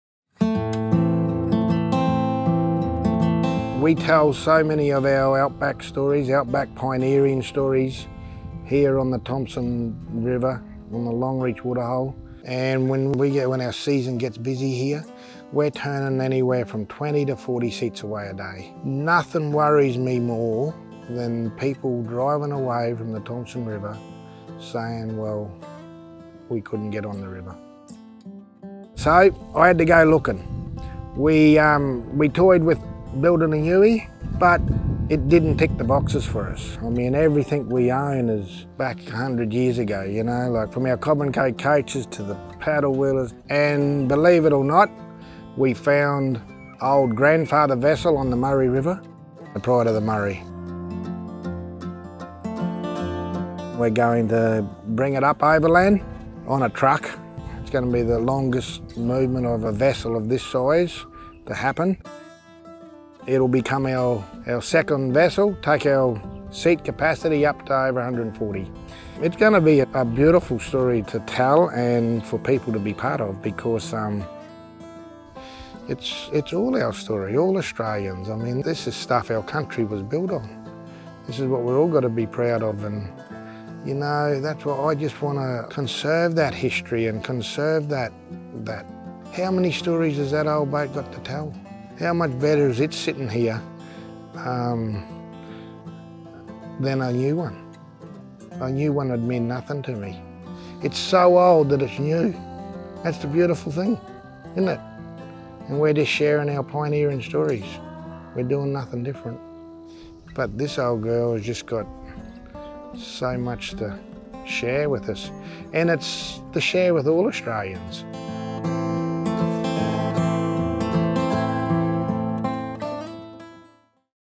The Pride of the Murray
With tales of her days as a wool-barge in Victoria in the 1920s, The Pride of the Murray brings new pioneer yarns to our wool heartland in Queensland as well as preserving an important part of Australia’s heritage. Hear Richard Kinnon, Founder, Outback Pioneers,  talk about his vision and the Pride of the Murray.